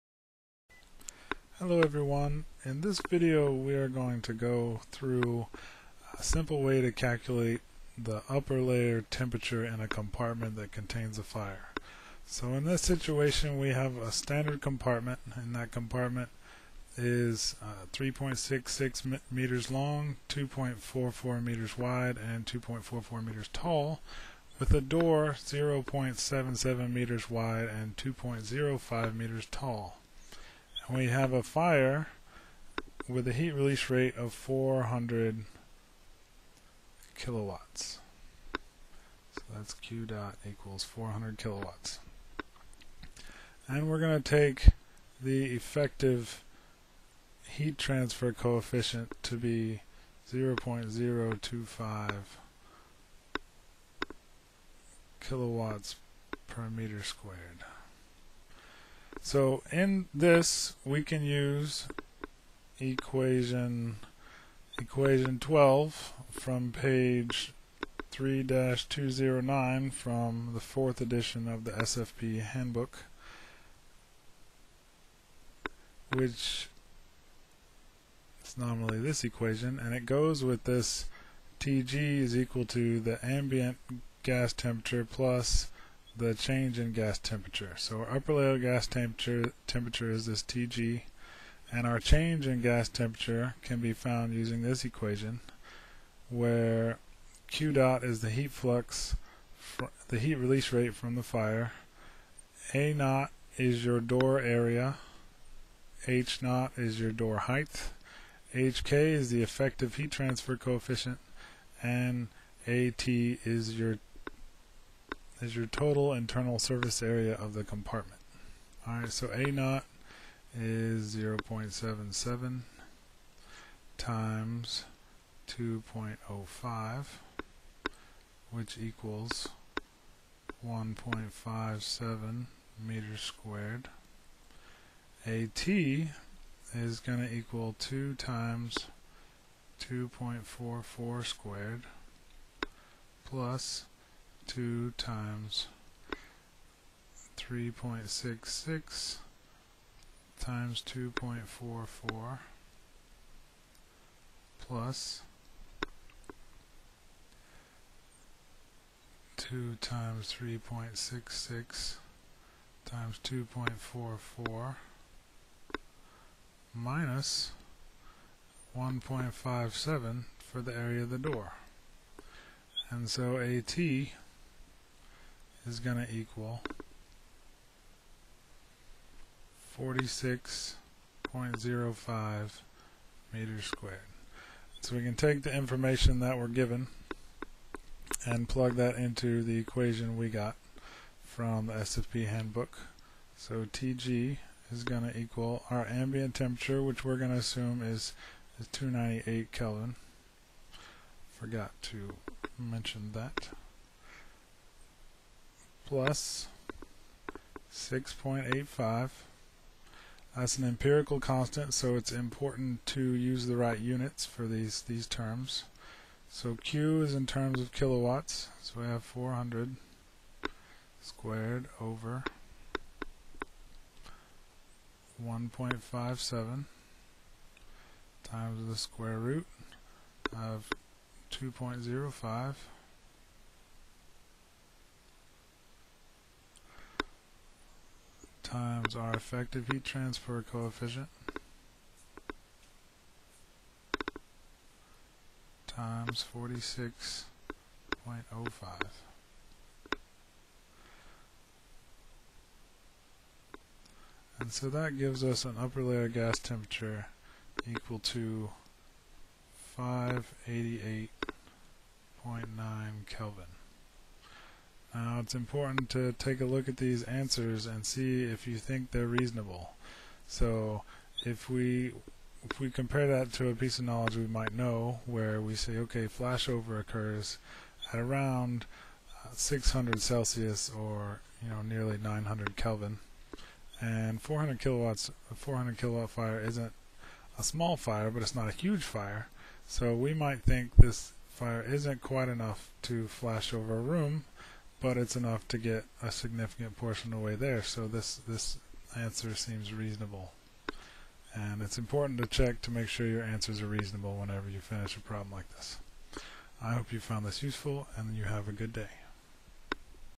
Upper Layer Gas Temperature in a Compartment Fire - FPE Teaching Tool
Example for how to calculate upper layer gas temperature assuming a 2 zone model using equation 12 from page 2-209 from the SFPE handbook 4th edition.